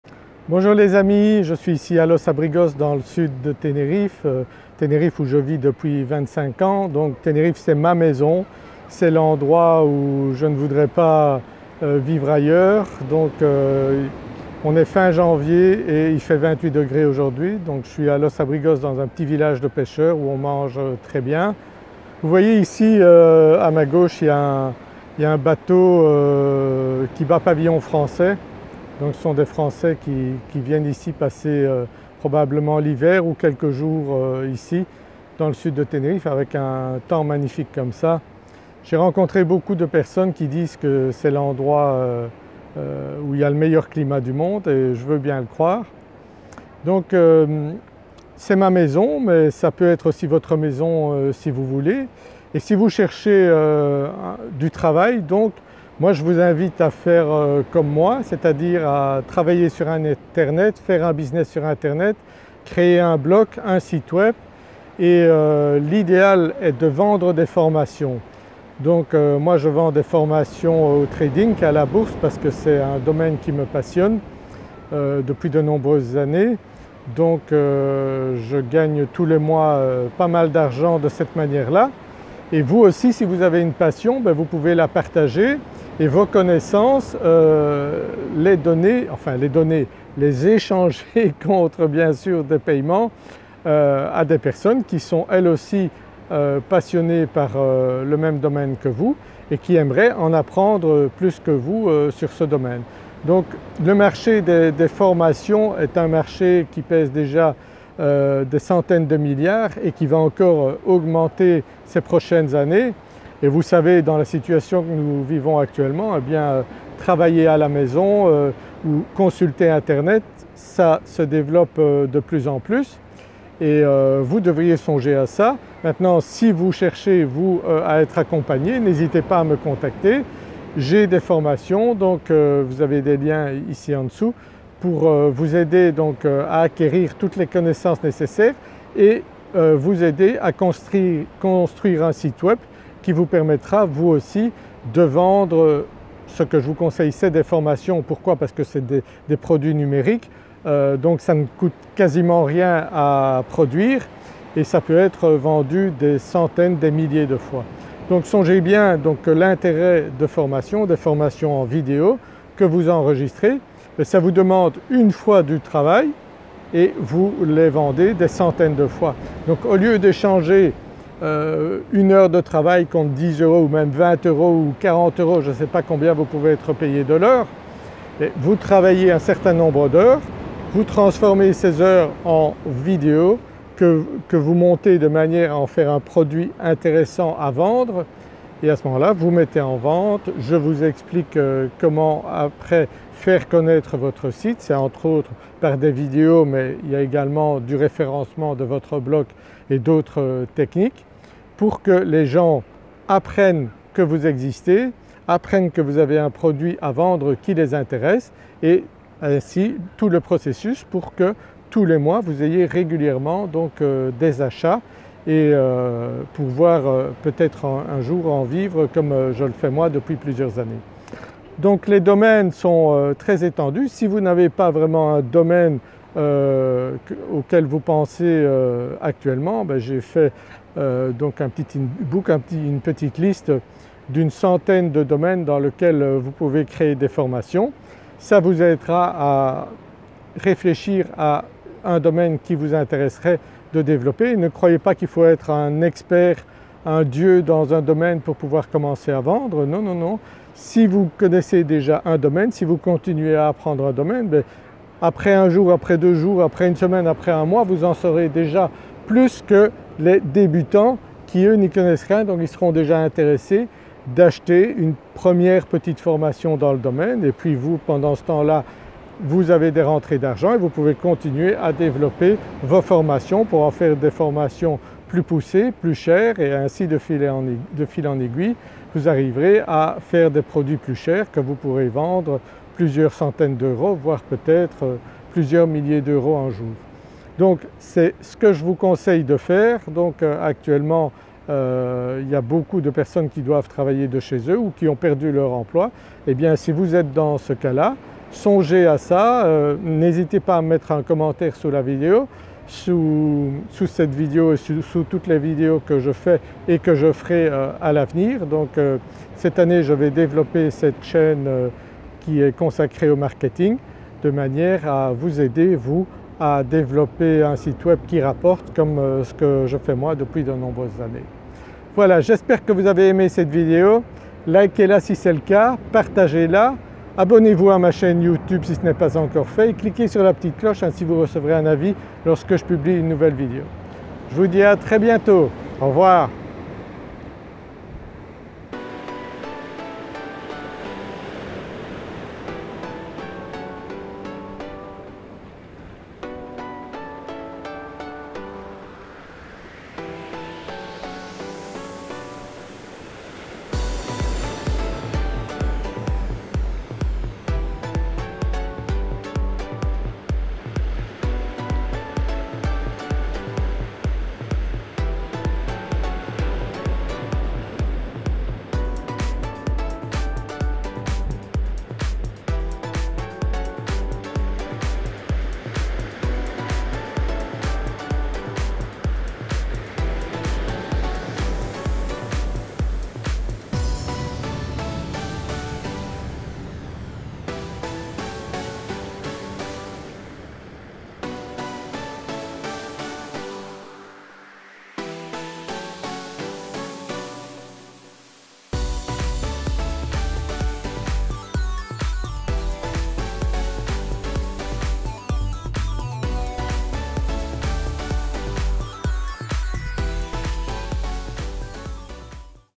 Le Secret Pour Gagner de l'Argent en Ligne Depuis Tenerife
Salut les amis ! 🌞 Imagine-toi vivre à Tenerife, profiter du climat parfait tout en gagnant de l'argent en ligne 💻. Aujourd'hui, je te dévoile comment je transforme ma passion en revenu grâce à la vente de formations sur internet.

🔑 **Formation Offerte**:
- Apprends à créer un business en ligne 🌐
- Découvrez les logiciels que j'utilise : Getresponse, Kooneo
- Optimise tes revenus avec des techniques de e-commerce

📈 **BOURSE - TRADING**:
- Mes stratégies pour trader efficacement 📊
- Comment j'ai commencé et comment tu peux aussi réussir !

🏡 **IMMOBILIER À TENERIFE**:
- Pourquoi Tenerife est l'endroit idéal pour vivre et investir
- Mon parcours et conseils pour te lancer 🌇

💬 **Commentaire**:
As-tu des questions ou besoin de conseils ? Laisse un commentaire, je suis là pour t'aider !

CHAPTERS:
0:00 - Vivre à Tenerife
2:06 - Pourquoi vendre des formations en ligne
3:38 - Vous n'avez pas de passion ?

▬▬▬▬▬▬▬▬▬▬▬▬▬ OUTILS ▬▬▬▬▬▬▬▬▬▬▬▬

bonus de 29 euros avec ce code : koo0b489f254